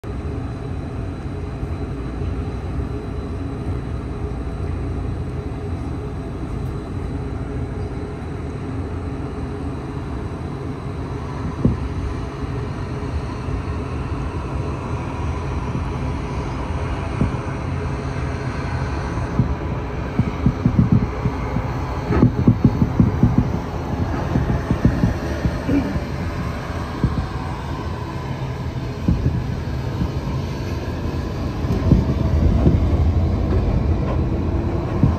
2024.11.5 台北松山機場 TSA/RCSS 立榮航空 ATR72-600（B-17016) B78809 (Taipei to Kinmen TSA 09:00 to KNH 10:20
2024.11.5 台北松山機場 TSA/RCSS 立榮航空 ATR72-600（B-17016) B78809 (Taipei to Kinmen TSA 09:00 to KNH 10:20